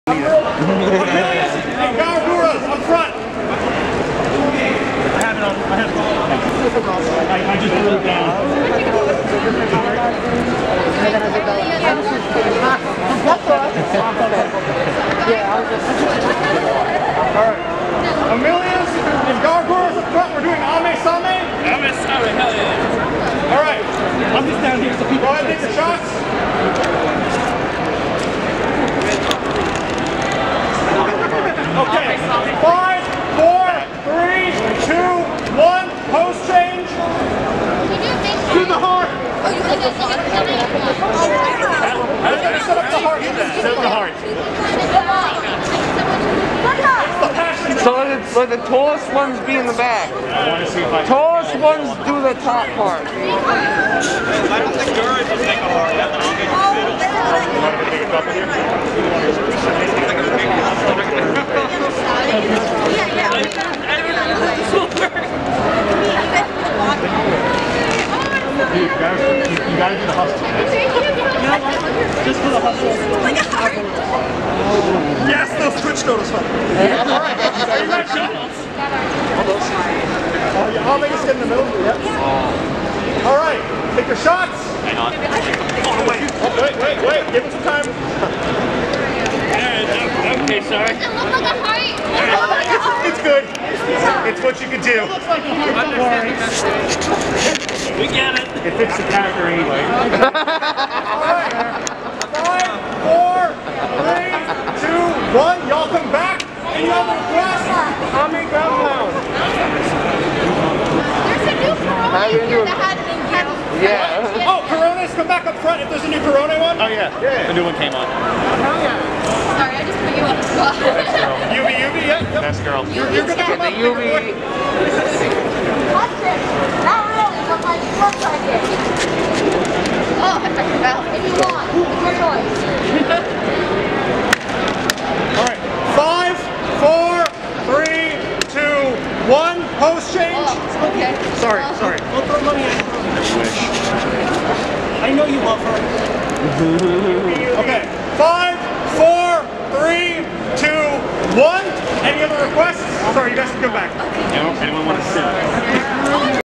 Attack On Gura Gawr Anime NYC 2021
Look at all those Hololive Gura, So many of them lol. Another part of the photoshoot. 800 more to go.

-Inugami Korone
-Gura Gawr
-Watson Amelia
Hope to see some Hololive X cosplay next year!